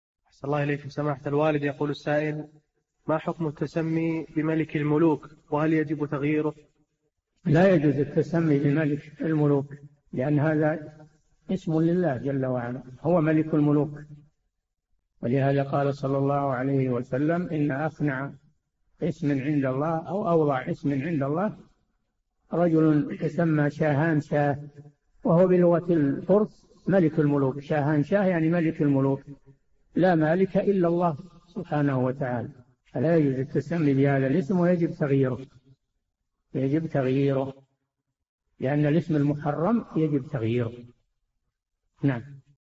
[1808- 4717] هل يجوز التسمي بملك الملوك وهل يجب تغييره؟ - الشيخ صالح الفوزان
[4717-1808] ما حكم التسمي بملك الملوك؟ وهل يجب تغييره؟
الجَوَابُ: لا يجوز التسمي بملك الملوك؛ لأن هذا اسم لله جل وعلا، هو ملك الملوك، ولهذا قال ﷺ «إن أخنع اسم عند الله أو أوضع عند الله رجل تسمى شاهان شاه» وهو بلغة الفرس ملك الملوك، شاهان شاه يعني ملك الملوك، لا مالك إلا الله سبحانه وتعالى، فلا يجوز التسمي بهذا الاسم ويجب تغييره، يجب تغييره؛ لأن الاسم المحرم يجب تغييره
السيرة الذاتية :
للاشتراك في قناة الشيخ صالح الفوزان
لمشاهدة حلقات فتاوى على الهواء كاملة

للاستفسار والإقتراحات والدعم
رئيس الفريق
لمشروع كبار العلماء